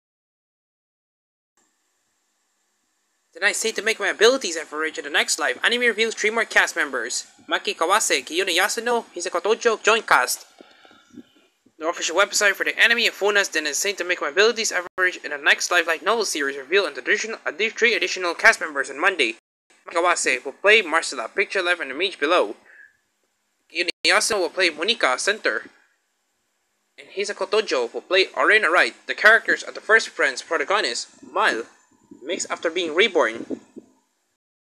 Didn't I Say to Make My Abilities Average in the Next Life?! Anime Reveals 3 More Cast Members
Maki Kawase, Kiyono Yasuno, Hisako Tōjō join cast